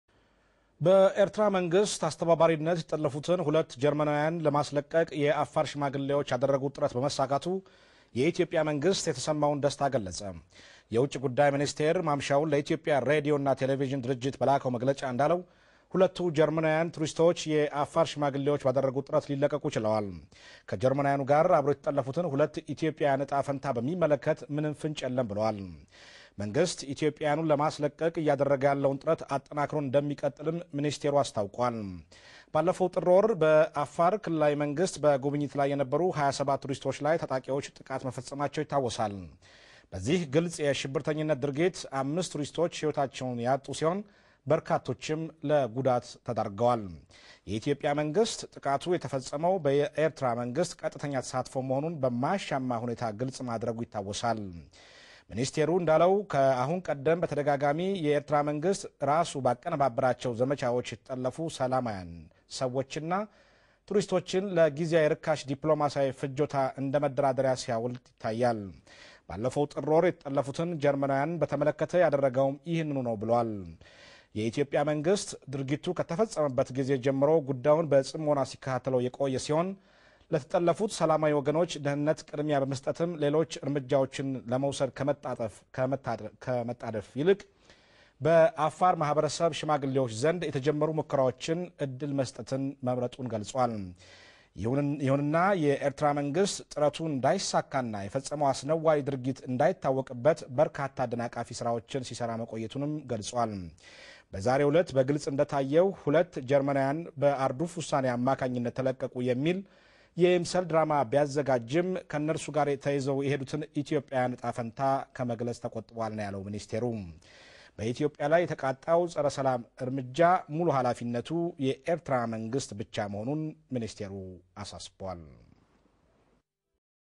2 German tourists kidnapped in Ethiopia released, official says
An Ethiopian official says two German tourists who were kidnapped during an attack by gunmen in January have been released.

Ethiopia's foreign ministry spokesman Dina Mufti said the Germans were freed from kidnappers who were associated with the country's archrival, Eritrea.